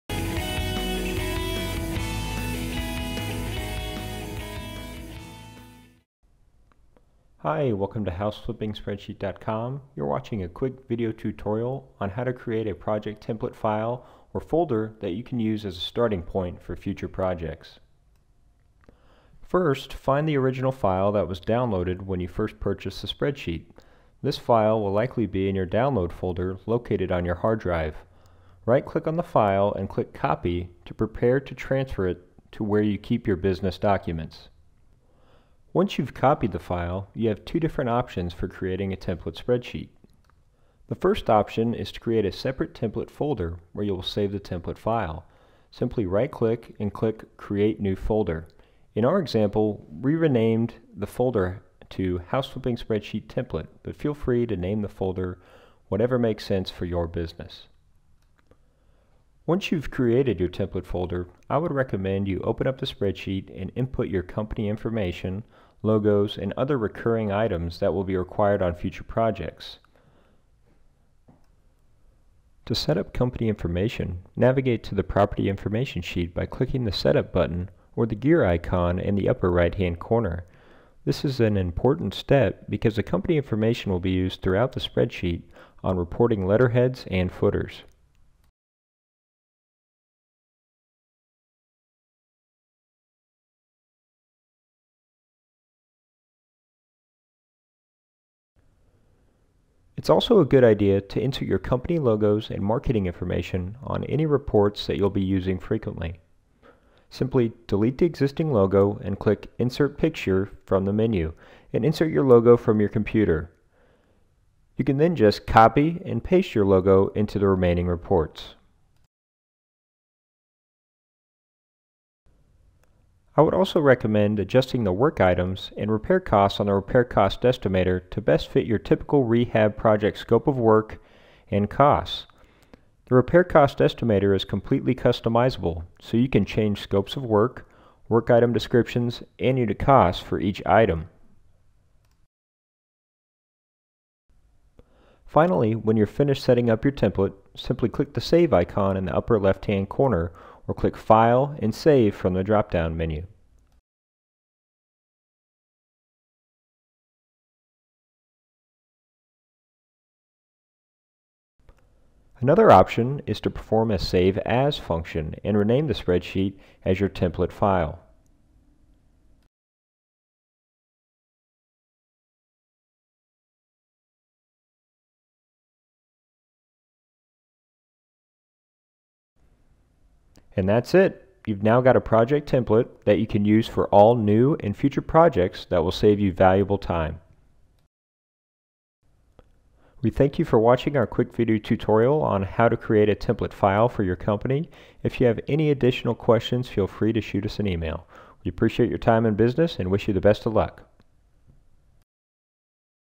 Getting Started - Creating a Template
Learn how to create a project template that can be used to save time for future projects and opportunities.